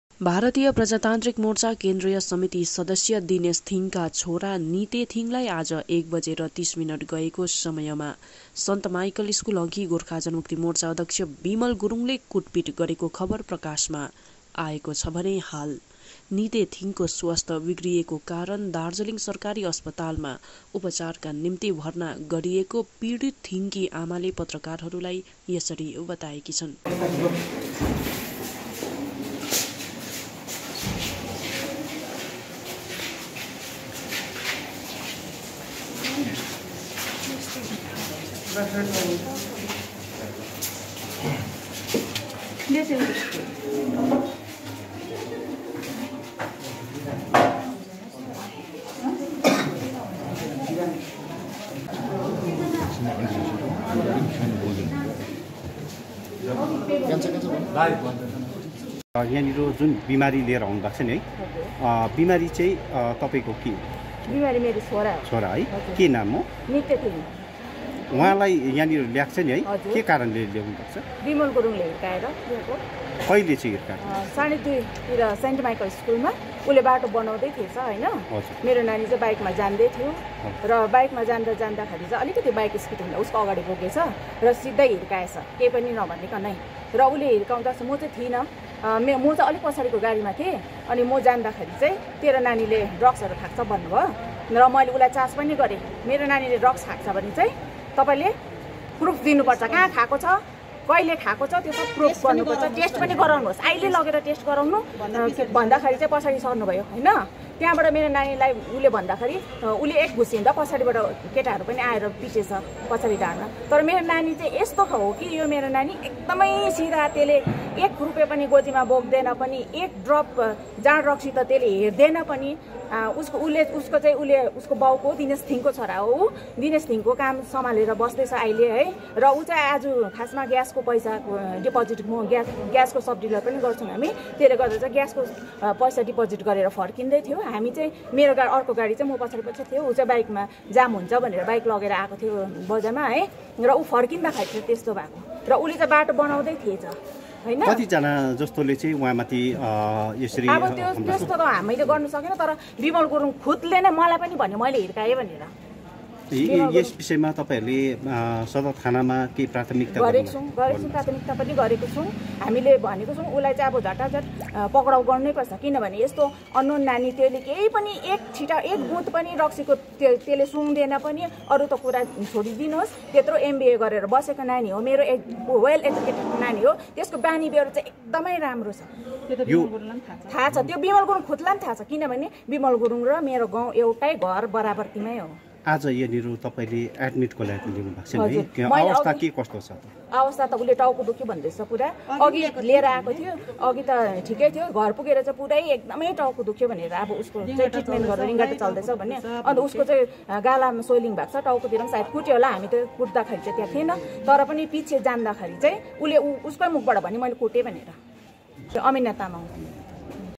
भारतीय प्रजातान्त्रिक मोर्चा केन्द्रीय समिति सदस्य दिनेश थिङका छोरा निते थिङलाई आज
भारतीय प्रजातान्त्रिक मोर्चा केन्द्रीय समिति सदस्य दिनेश थिङका छोरा निते थिङलाई आज एक बजेर तीस मिनट गएको समयमा सन्तमाइकल स्कूल अघि गोर्खा जनमुक्ति मोर्चा अध्यक्ष बिमल गुरुङले कुटपिट गरेको खबर प्रकाशमा आएको छ भने हाल निते थिङको स्वास्थ्य बिग्रिएको कारण दार्जीलिङ सरकारी अस्पतालमा उपचारका निम्ति  भर्ना गरिएको पिडित थिङकी आमाले पत्रकारहरुलाई यसरी जानकारी दिएकी छन् ।